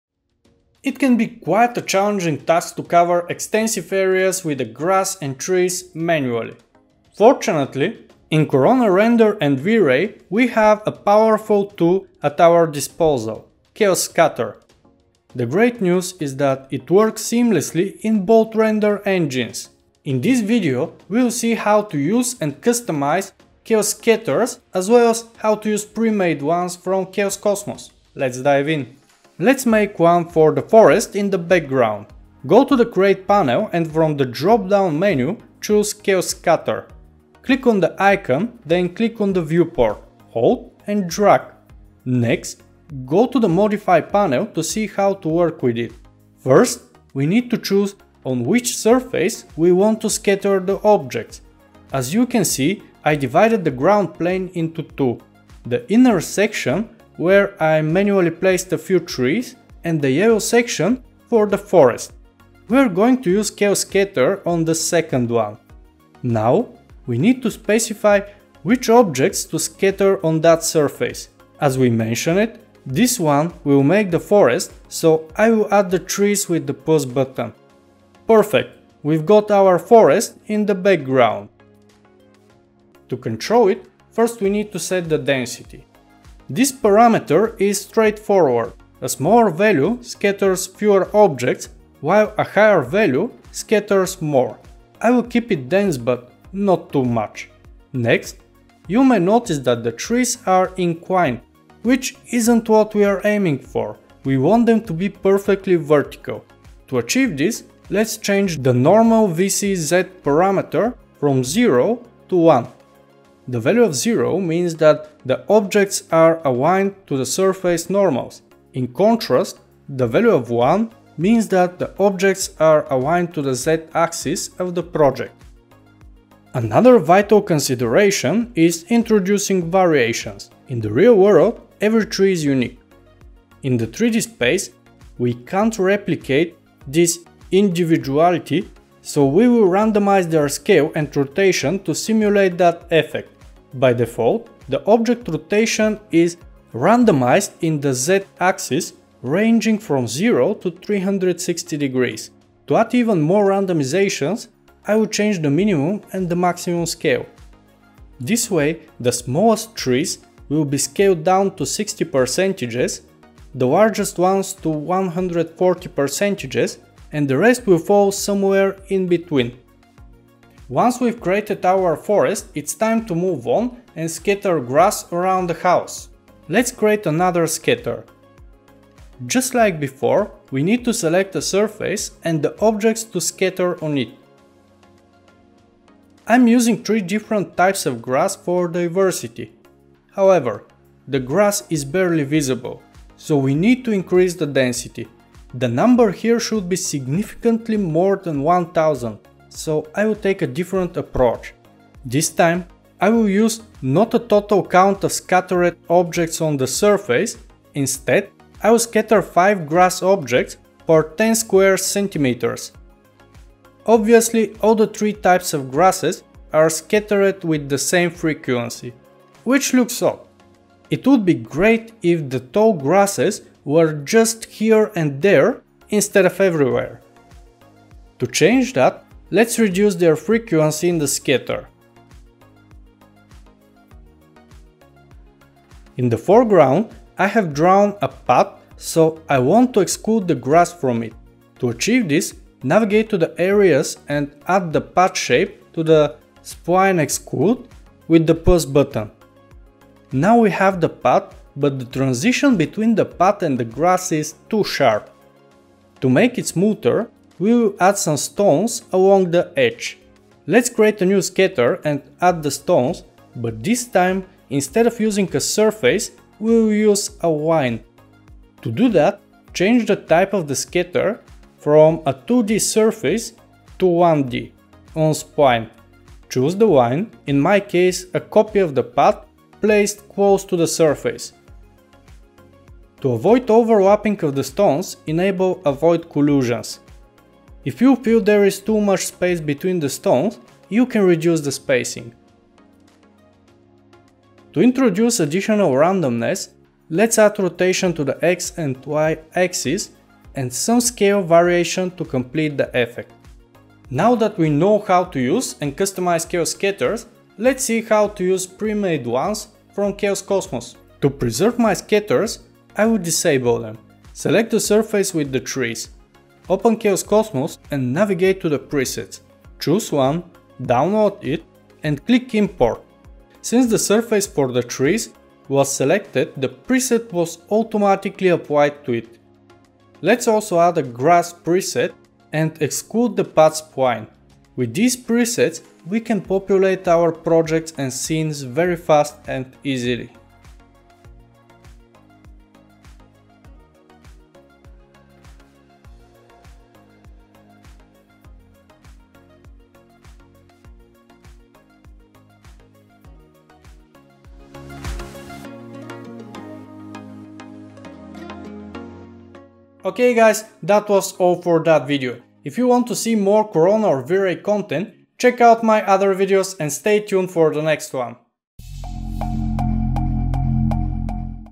Add grass and trees easily! / Chaos Scatter
Join me in this tutorial and learn how to use Chaos Scatter!